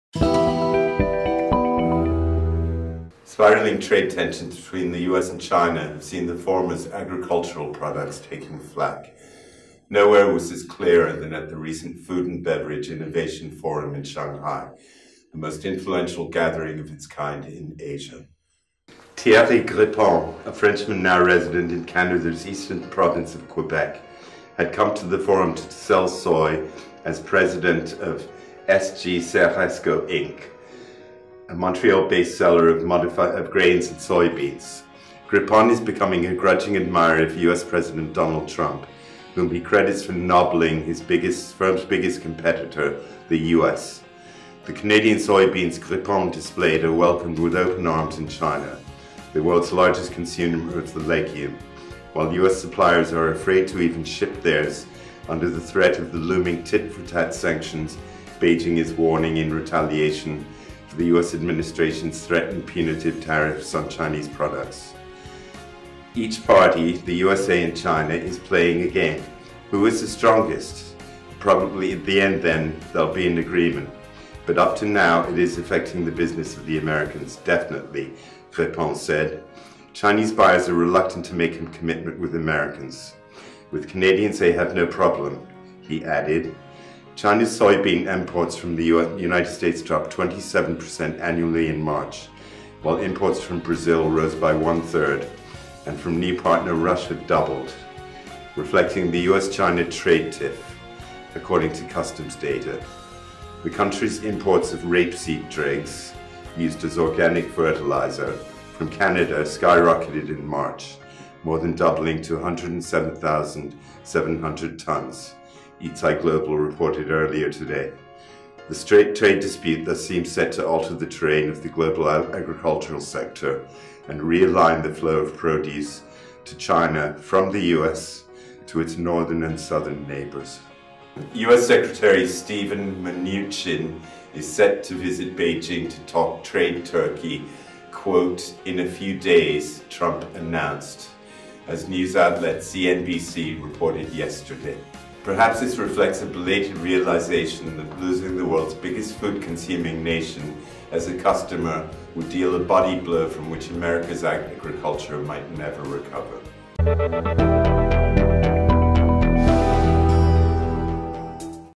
Trade Tiff Threatens to Transform China’s Foreign Food Sourcing
Spiraling trade tensions between the US and China have seen the former’s agricultural products taking flak.